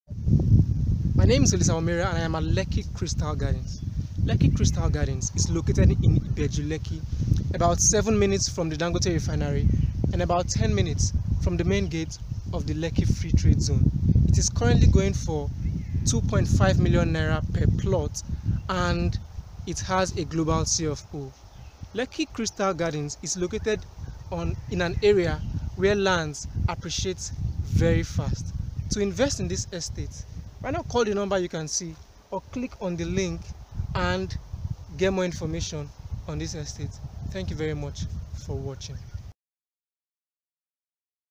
Lekki Crystal Garden Ibeju Lekki
Lekki Crystal Garden is located in ibeju lekki, about 7 minutes from the main gate of the lekki free trade zone and about  5 minutes from the dangote refinery.

Its location and proximity to the free trade zone makes it a good choice for investment